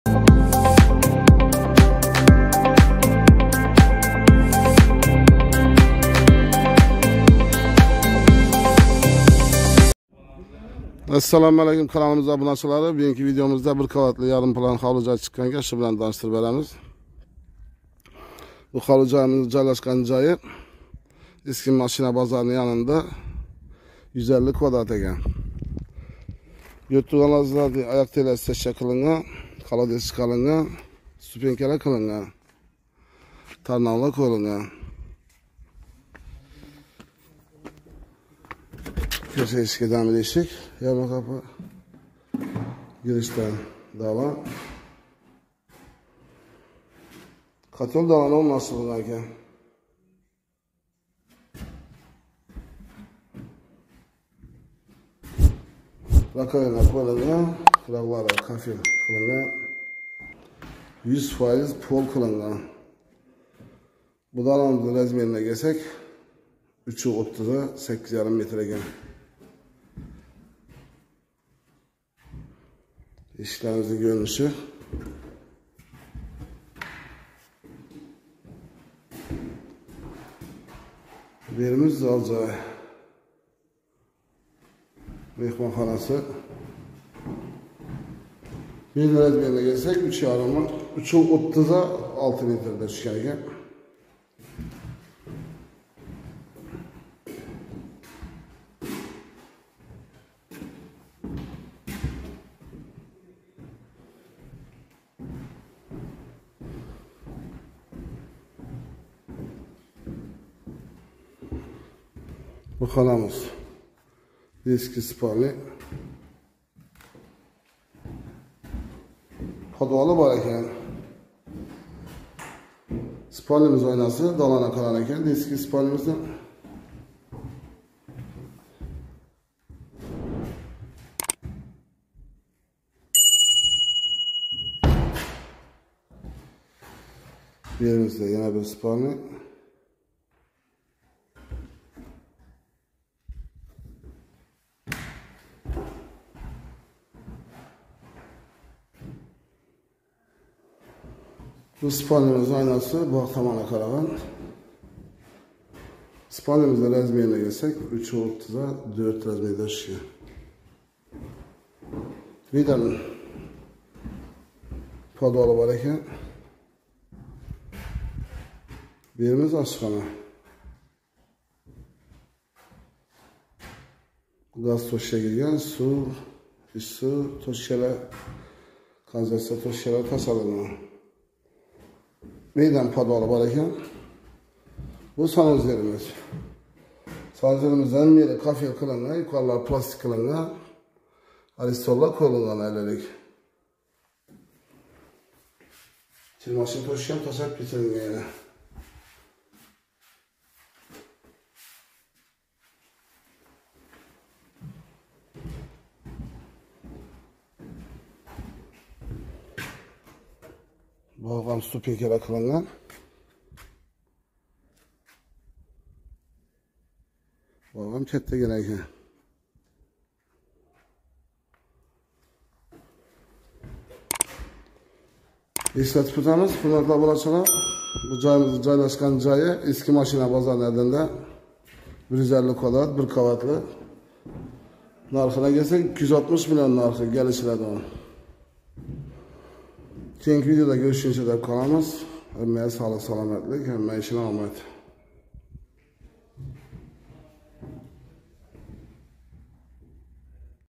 1 каватли 150 м2 ховли/ Урганч/. Мулжал: Эски машина бозори.
📍УРГАНЧ  ШАХРИДА
🏠 1 КАВАТЛИ  150 М2
💰 НАРХИ  260 МЛН
🏛 МУЛЖАЛ  ЭСКИ МАШИНА БОЗОРИ
⏰️ ОНЛАЙН  24/7 СОАТ  ИШ ФАОЛИЯТИ
🏃 СИЗ  УЧУН ДОИМ ИЗЛАНИШДАМИЗ
👍 ТАНЛОВДА АДАШМАНГ
🏗 КАХРАМОН УЙ ЖОЙ КАНАЛИ